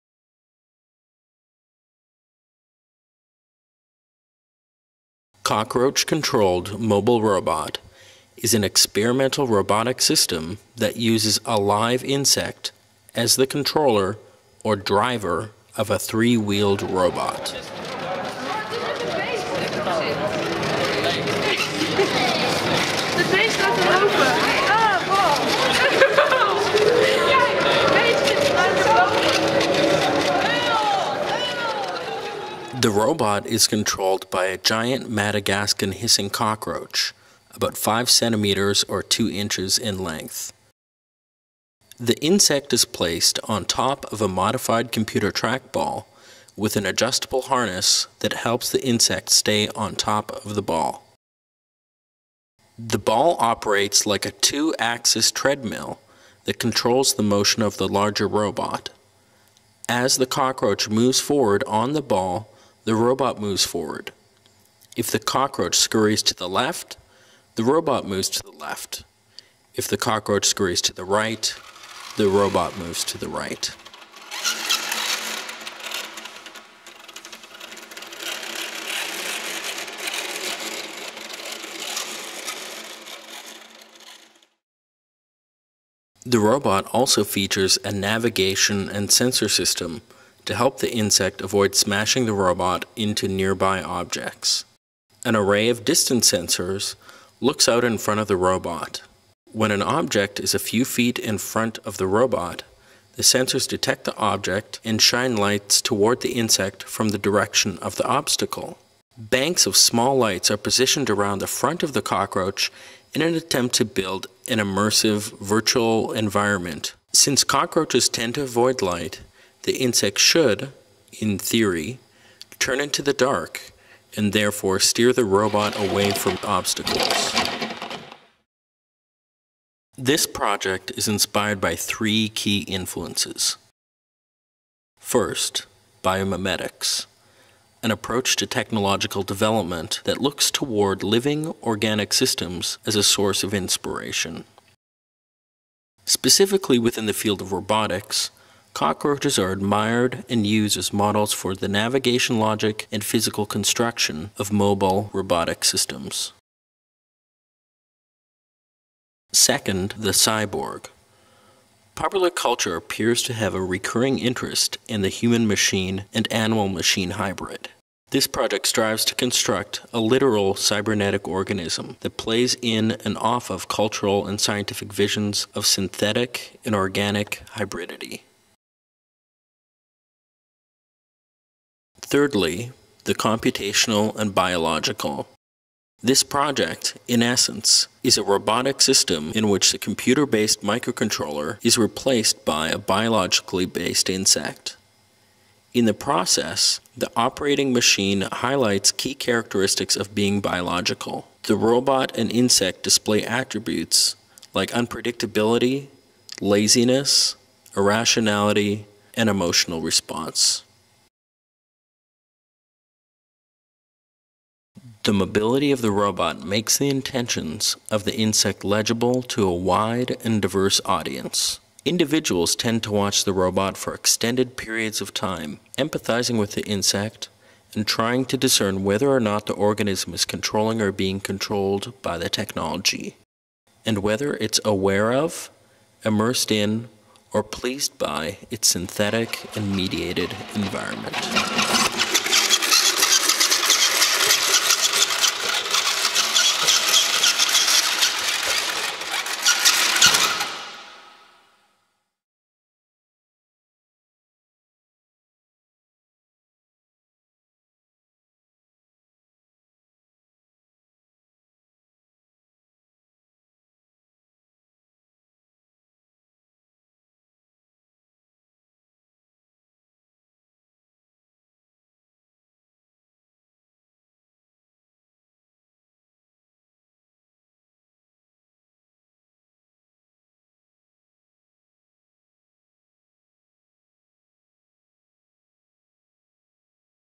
The Cockroach Controlled Mobile Robot (Higher Resolution)
- "Cockroach Controlled Mobile Robot" is an experimental robotic system that uses a live insect as the controller, or driver, of a three-wheeled robot.
[00:30]  Robot Control
The system is controlled by a Giant Madagascan Hissing Cockroach, about 5 cm, or 2 inches, in length.  The insect is placed on top of a modified computer trackball, with an adjustable harness that helps the insect stay on top of the ball.  The ball operates like a two axis treadmill that controls the motion of the larger robot: as the cockroach moves forward on the ball, the robot moves forward.  If the cockroach scurries to the left, the robot moves to the left.  If the cockroach scurries to the right, the robot moves to the right.
[01:25]  Sensors & Feedback
The robot also features a navigation and sensor system to help the insect avoid smashing the robot into nearby objects.  An array of distance sensors looks out in front of the robot.  When an object is a few feet in front of the robot, the sensors detect the object and shine lights toward the insect from the direction of the obstacle.
Banks of small lights are positioned around the front of the cockroach in attempt to build an immersive, virtual environment.  Since cockroaches tend to avoid light, the insect should -- in theory -- turn into the dark and therefore steer the robot away from obstacles.
[02:09]  Motivations
This project is inspired by three key influences.
[02:15]  Biomimetics
First:. Biomimetics. an approach to technological development that looks toward living, organic systems as a source of inspiration.  Specifically within the field of robotics, cockroaches are admired and used as models for the navigation logic and physical construction of mobile robotic systems.
[02:40]  Cyborg
Second: The Cyborg. Popular culture appears to have a recurring interest in the human-machine and animal-machine hybrid.  This project strives to construct a literal cybernetic organism that plays in and off of cultural and scientific visions of sythetic and organic hybridity.
[03:03]  Computational/Biological
Thirdly, the Computational and Biological.  This project, in essence, is a robotic system in which the computer based microcontroller is replaced with a biologically-based insect. In the process, the operating machine highlights key characteristics of being biological.  The robot and insect display attributes like unpredictability, laziness, irrationality and emotional response.
[03:32]  Audience Response
The mobility of the robot makes the intentions of the insect legible to a wide and diverse audience.  Individuals tend to watch the robot for extended periods of time, empathizing with the insect, and trying to discern whether or not the organism is controlling or being controlled by the technology... and whether it is aware of, immersed in, or pleased by its synthetic and mediated environment.
[04:15]  Garnet Hertz 2006
[04:20]